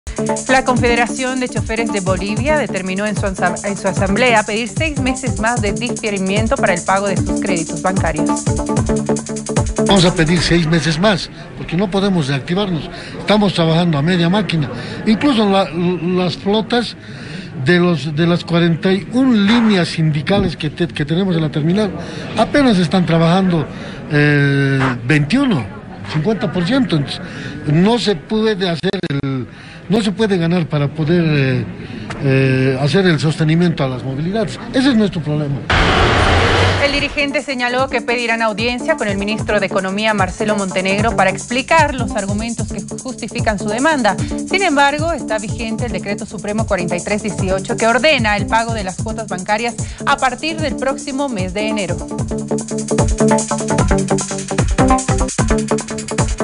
Choferes pide que se amplíe por seis meses el diferimiento de créditos
La Confederación de Choferes de Bolivia, en una asamblea, resolvió pedir al Gobierno la ampliación por seis meses más el diferimiento de créditos bancarios. Argumentan que su sector aún no se reactivó y no cuentan con recursos para cumplir con sus obligaciones bancarias.